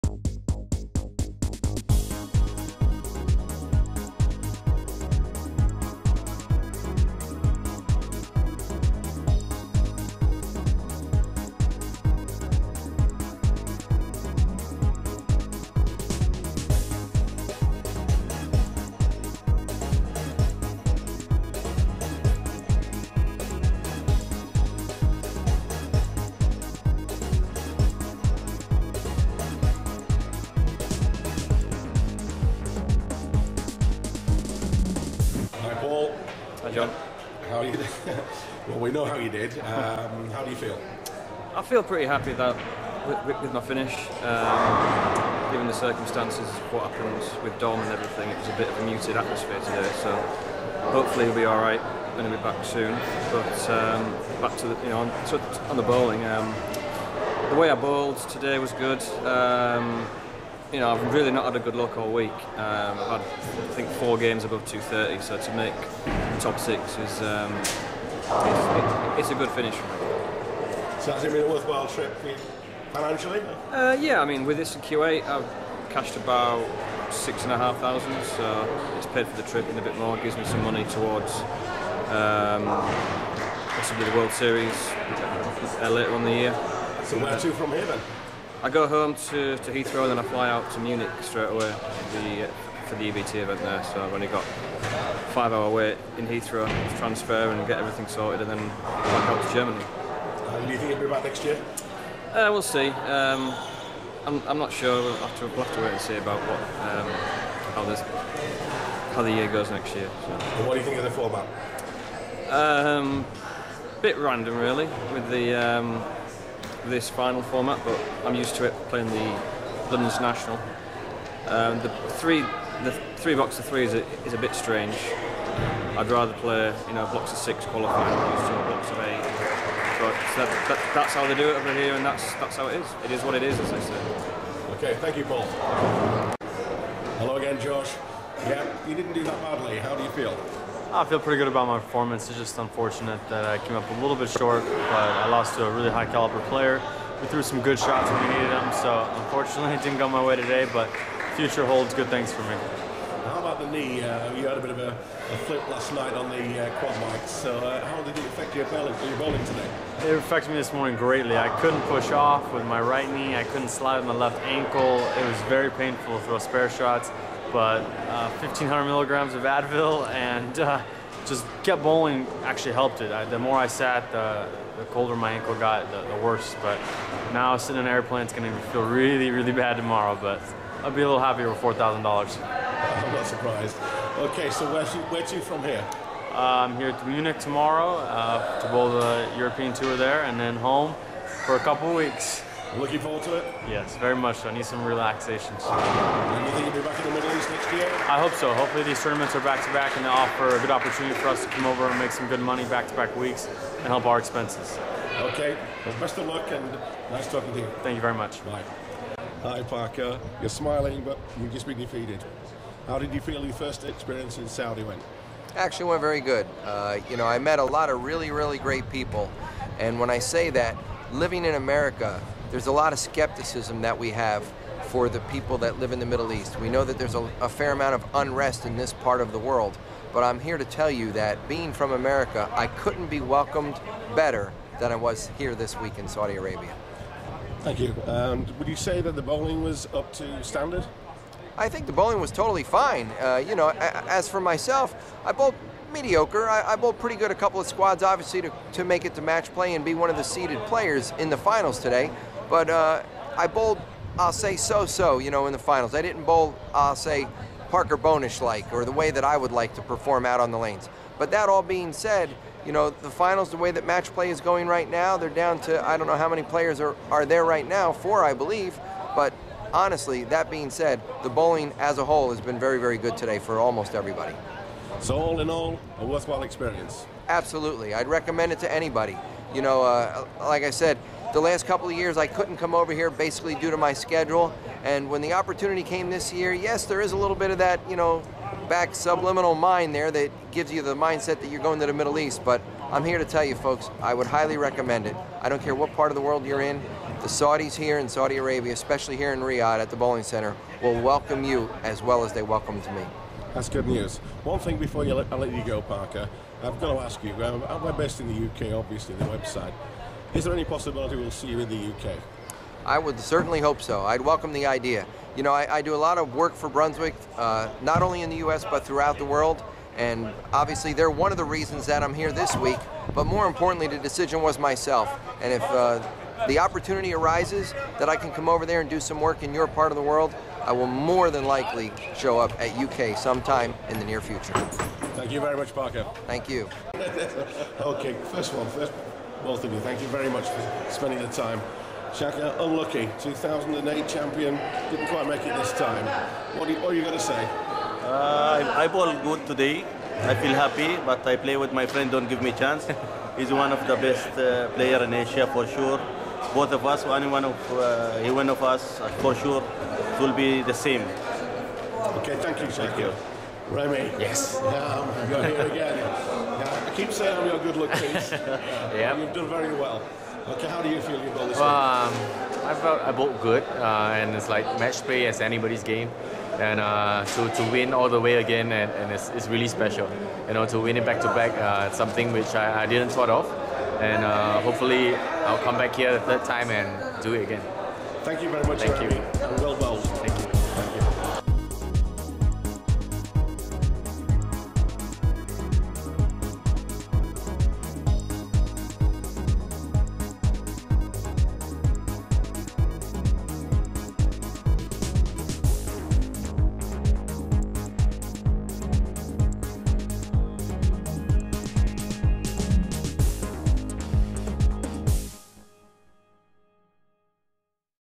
Saudi Open 2011 - Post Tournament Interviews
Interviews with Paul Moor, Josh Blanchard, Parker Bohn III and the finalists 2008 champion Shaker al-Hassan (UAE) and back-to-back 2010 and 2011 champion Remy Ong of Singapore following the conclusion of the 5th Kingdom International Open Bowling Tournament (2011 Saudi Open) which ran from the 24th to 30th March 2011 at the Universal Bowling Center in Riyadh, Saudi Arabia.